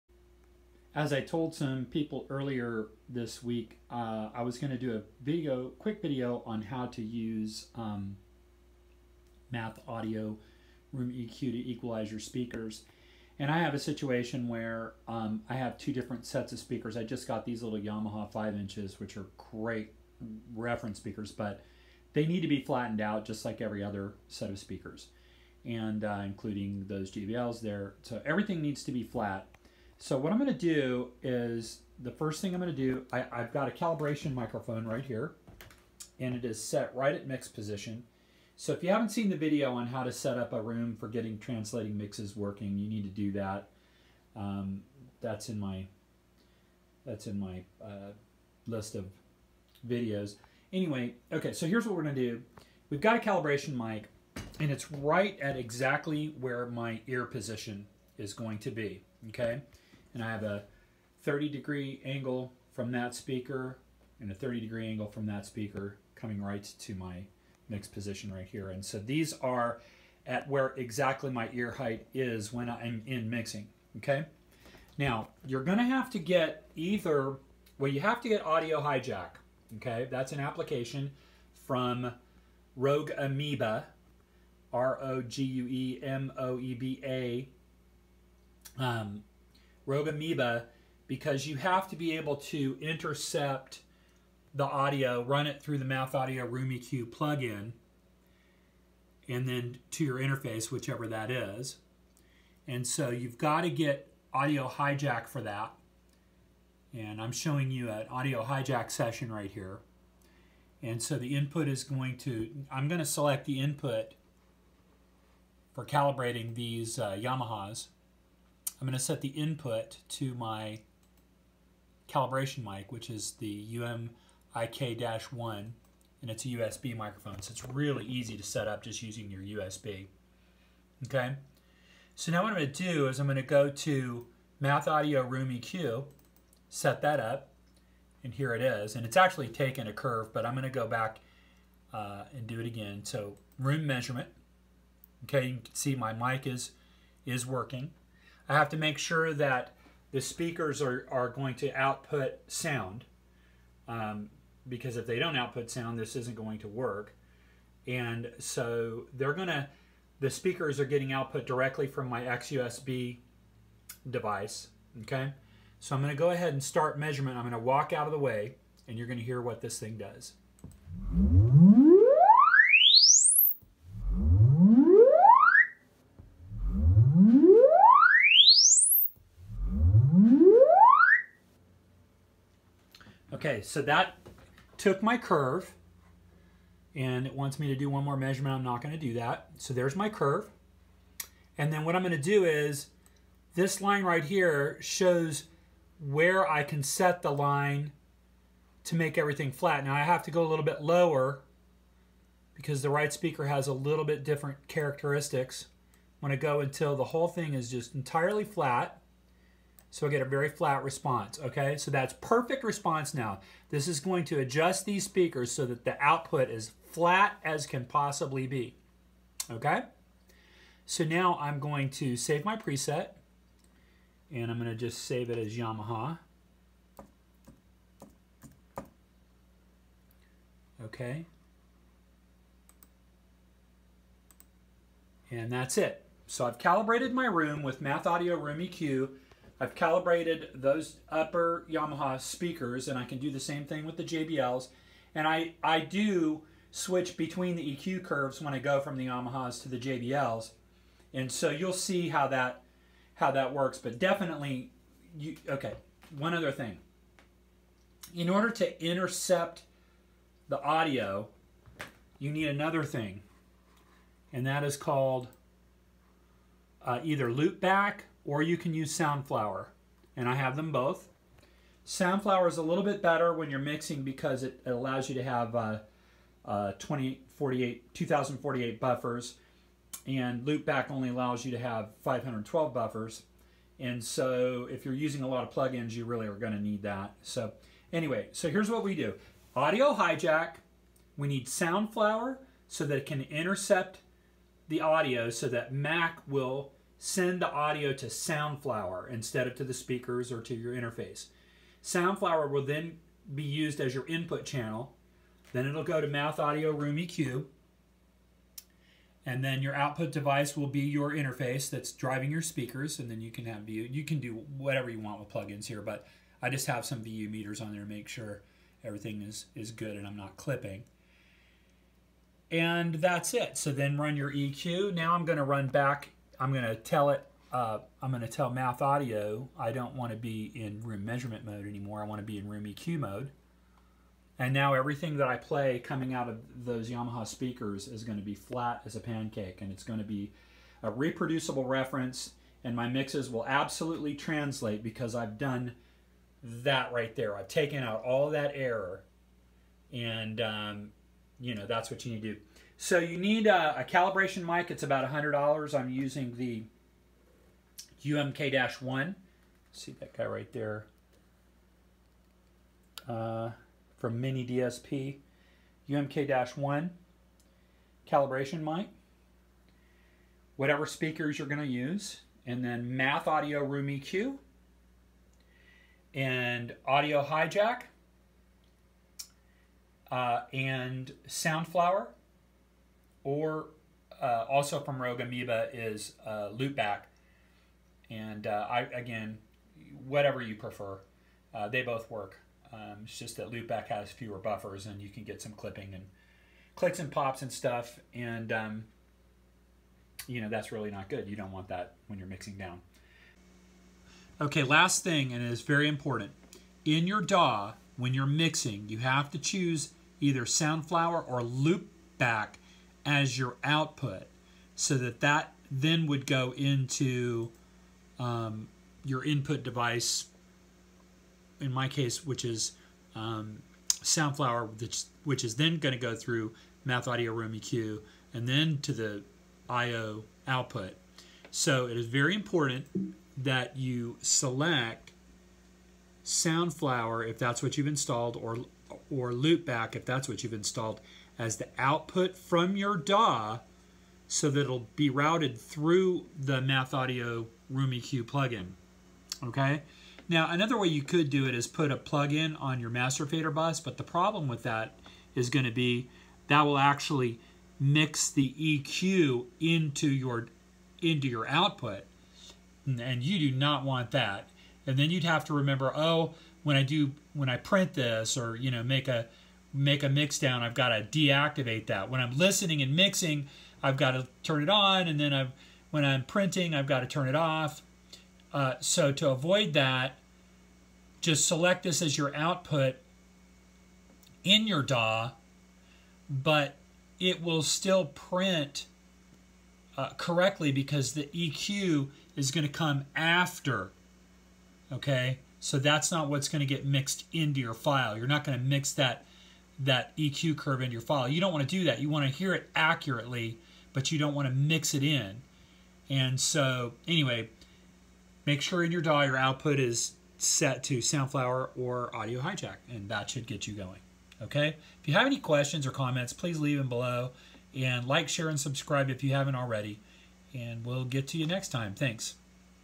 Using MathAudio RoomEQ in Mixing Environment
Mixing with a correct EQ is absolutely essential for getting mixes to translate.  After all of your room treatment is properly done.  The last step is to use an EQ correction plugin.  I really like MathAudio RoomEQ.  It does the trick in a huge way!